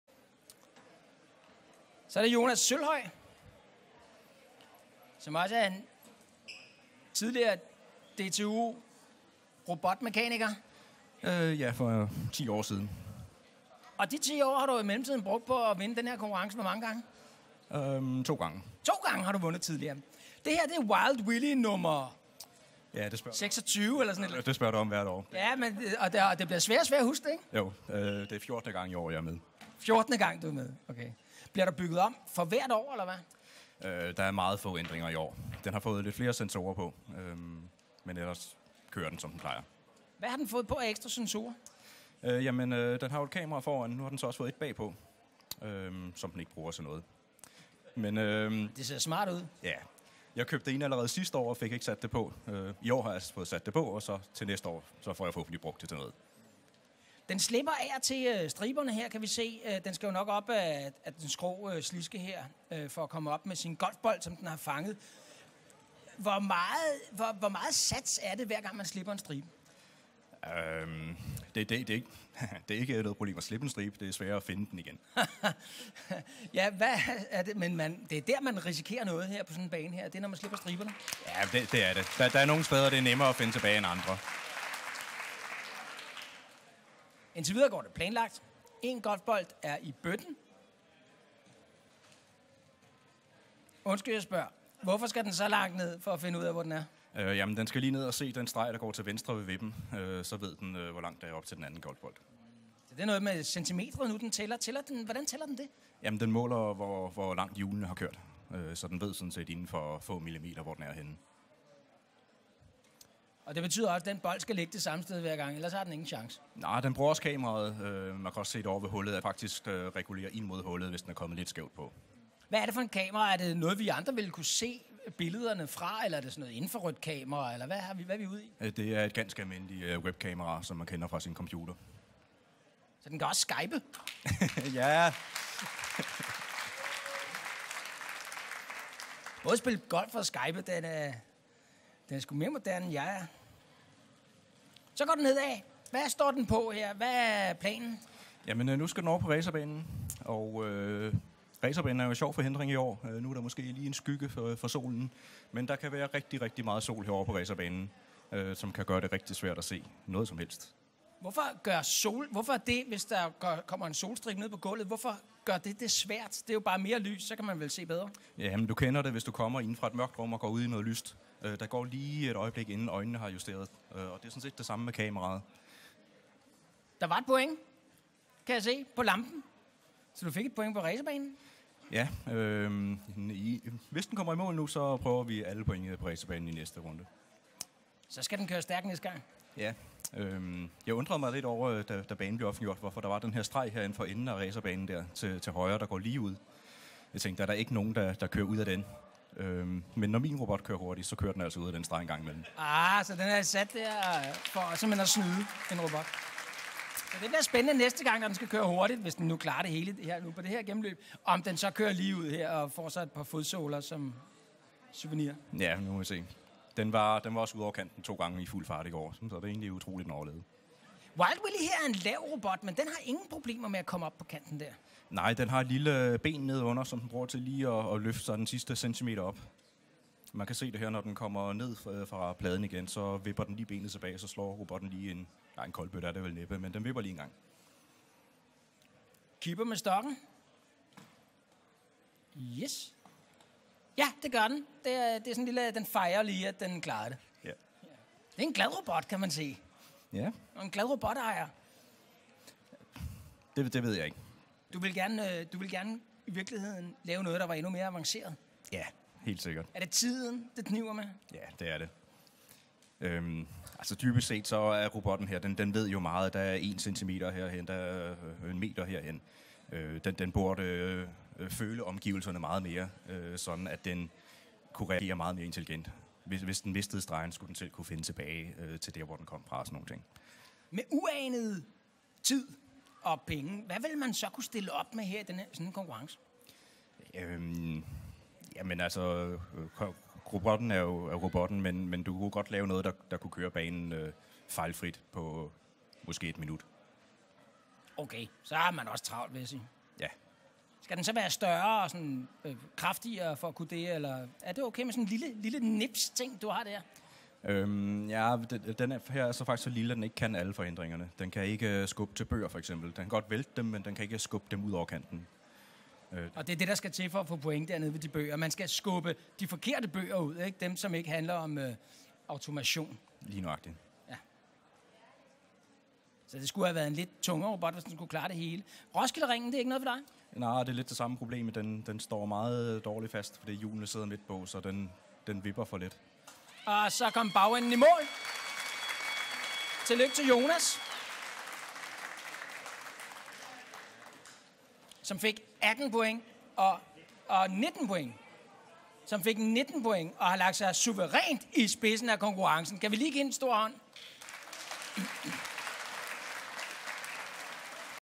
DTU RoboCup 2015 - Wild Willy - First run
First run in the final - 19 points.